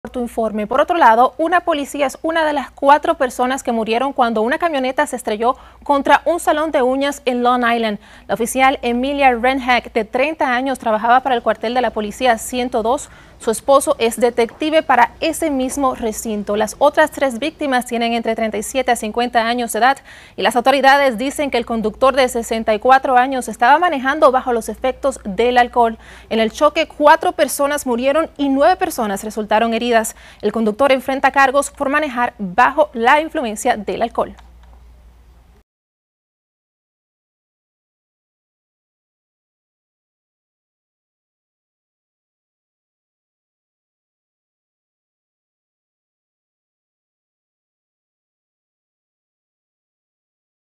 Oficial de NYPD, entre 4 personas muertas en choque contra salón de uñas
Emilia Rennhack, de 30 años, trabajaba para el Departamento de la Policía de Nueva York (NYPD) y es una de las cuatro personas que murieron luego de que un conductor ebrio se estrellara contra un negocio de manicura en Long Island.